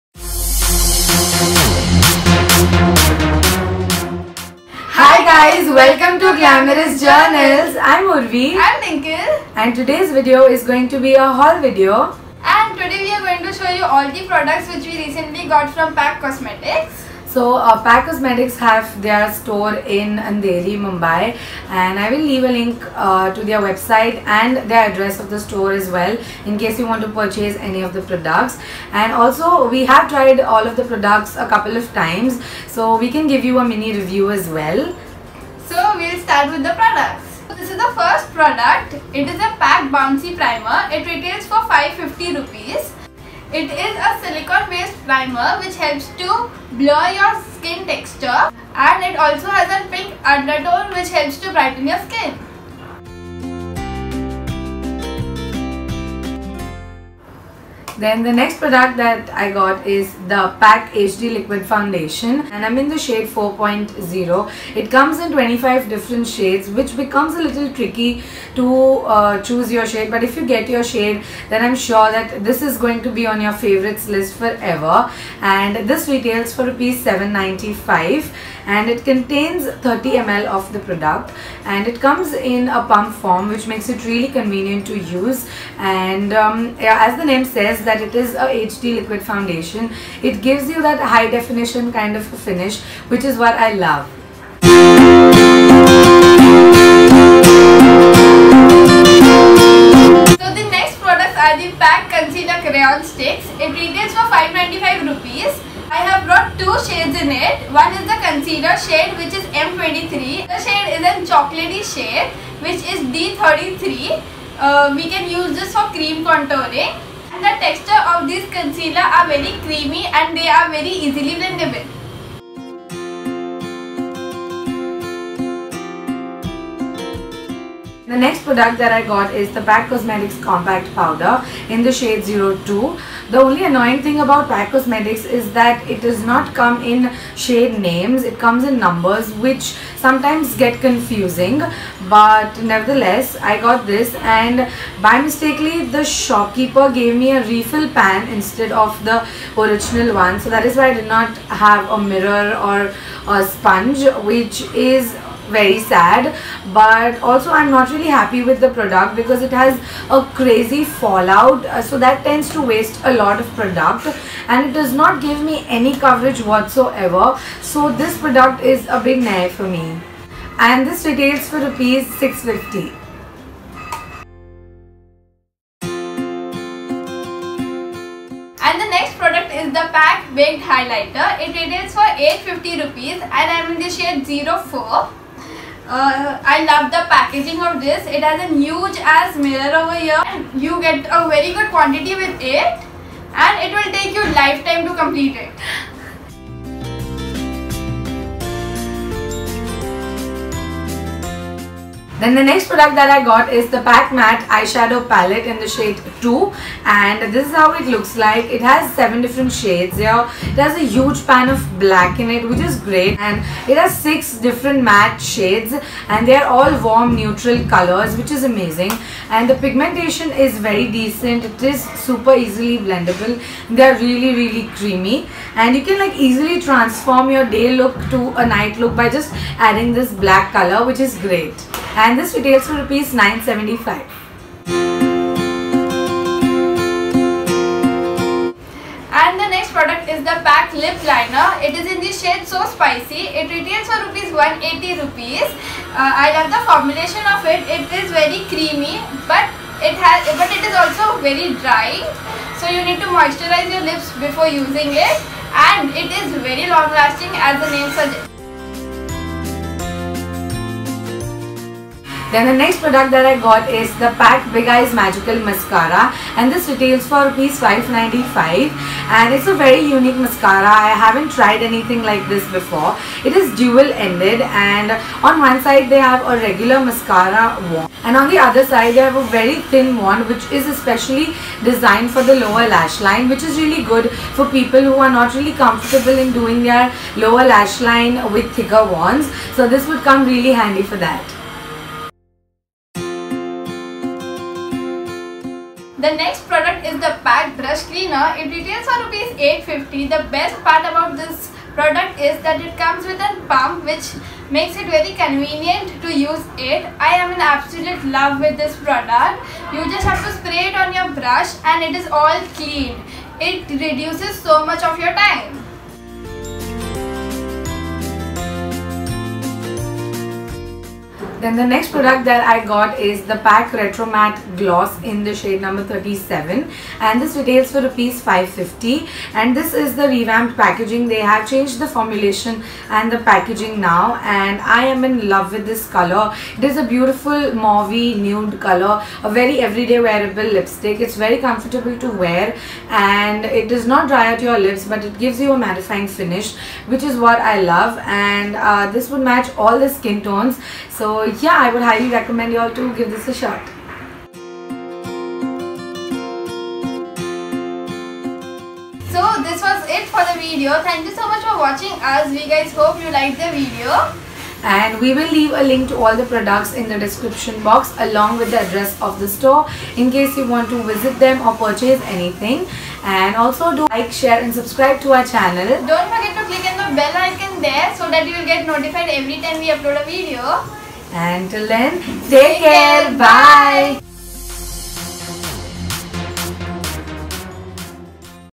PAC HAUL | Glamorous Journals
Heyaa guys..Its Urvi & Dinkle here.
We recently visited the Pac Cosmetics store in Mumbai & just could not resist shopping! So we picked quite a lot of stuff and also thought of sharing it with you guys.
XOXO.
You can also visit their store :
SHOP NO-01, WOODROW BLDG, VEERA DESAI ROAD , OPP POLICE STATION, ANDHERI (WEST), Industrial Area, Andheri West, Mumbai, Maharashtra 400053